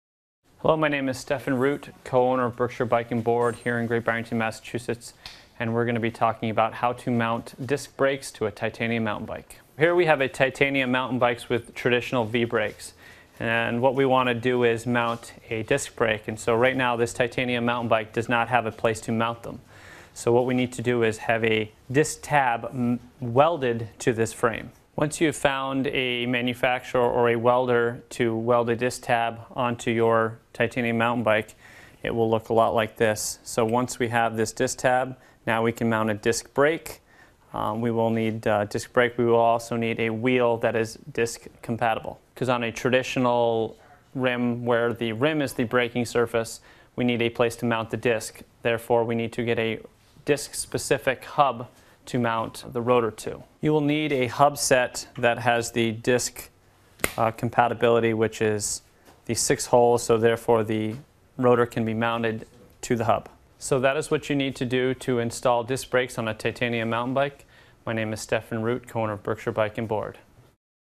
How to Mount Rear Disc Brakes on Titanium Mountain Bikes
How to Mount Rear Disc Brakes on Titanium Mountain Bikes. Part of the series: BMX & Mountain Bikes. You have to follow a very strict procedure if you want your rear disc brakes to work properly. Learn how to mount rear disc brakes on a titanium mountain bike with help from the co-owner of Berkshire Bike and Board in this free video clip.